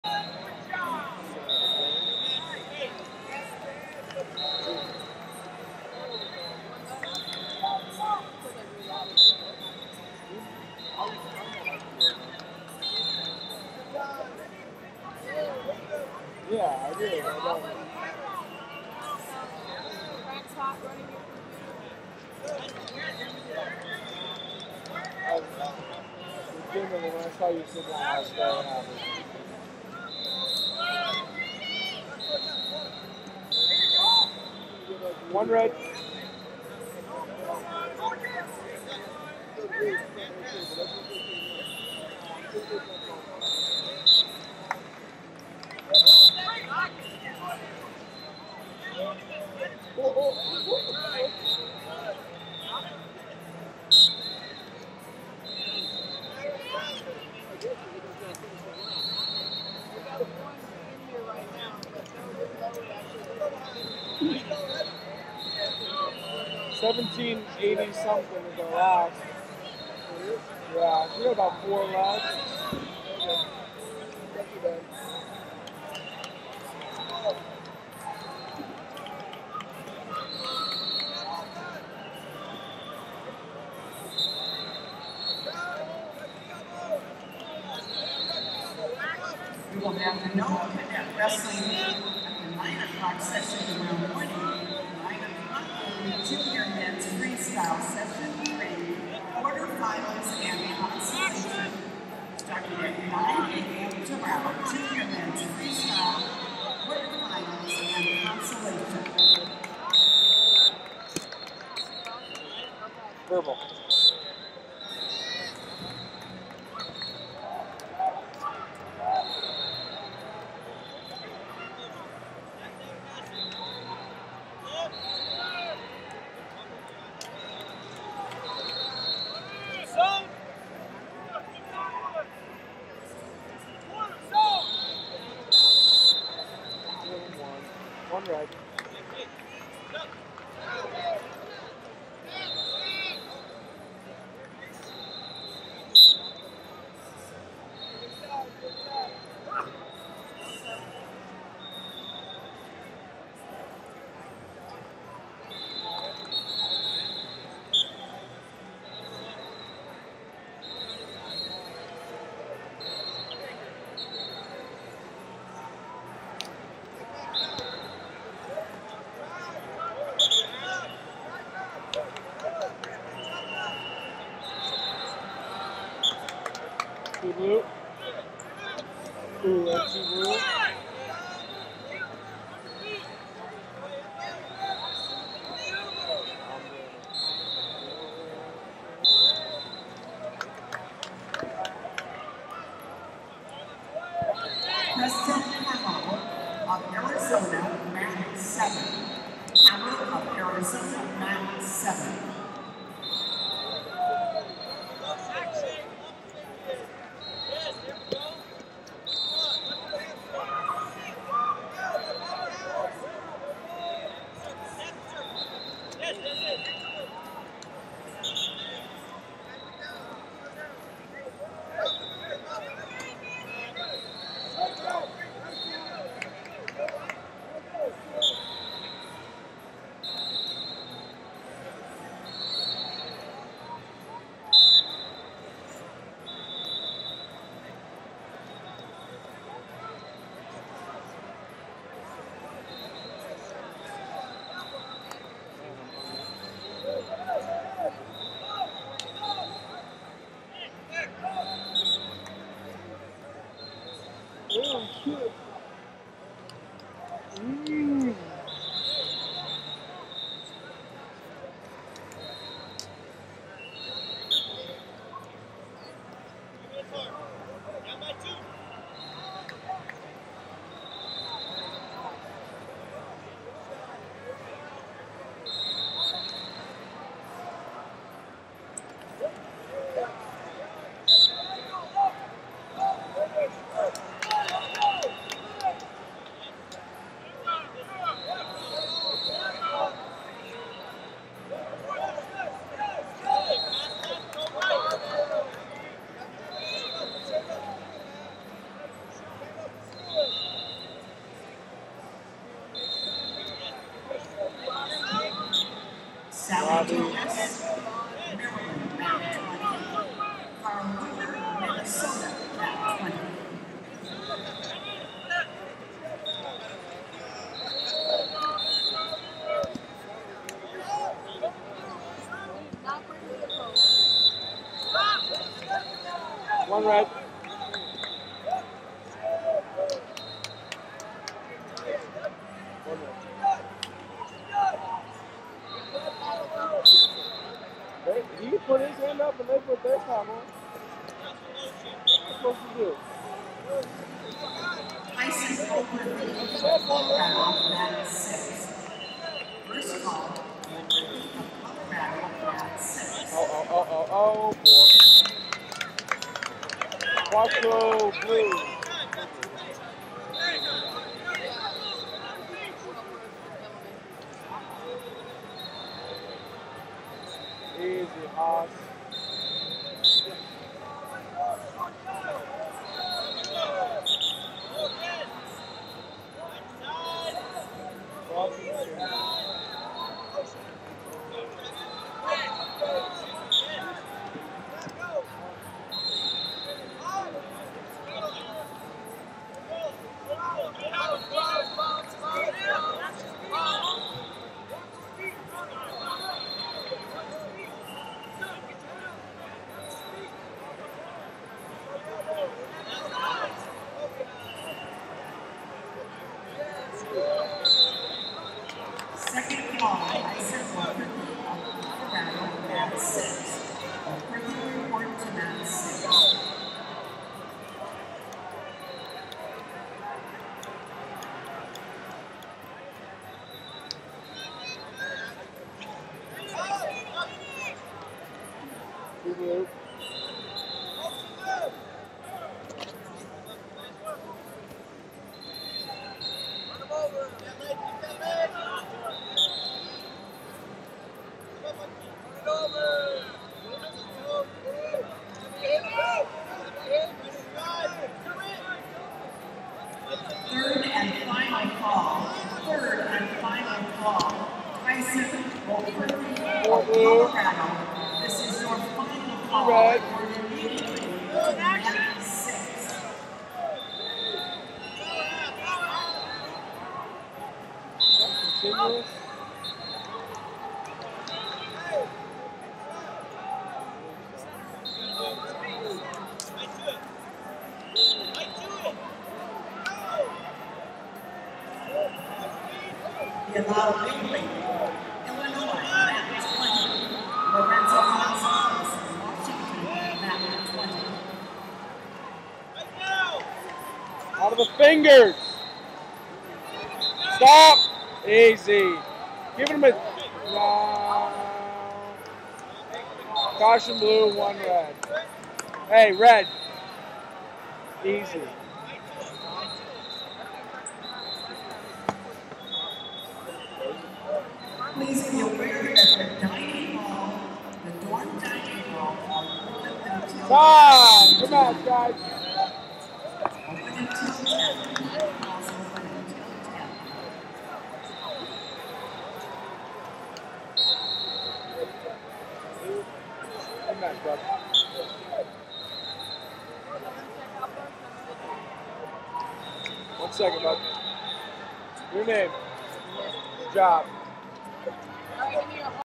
138 Lbs Cons 32 2 Matthew Zuckerman Ohio Vs Jalen Spuhler Wisconsin
2019 USMC USAW Junior And Cadet National Championships - 138 Lbs Cons 32 2 Matthew Zuckerman Ohio Vs Jalen Spuhler Wisconsin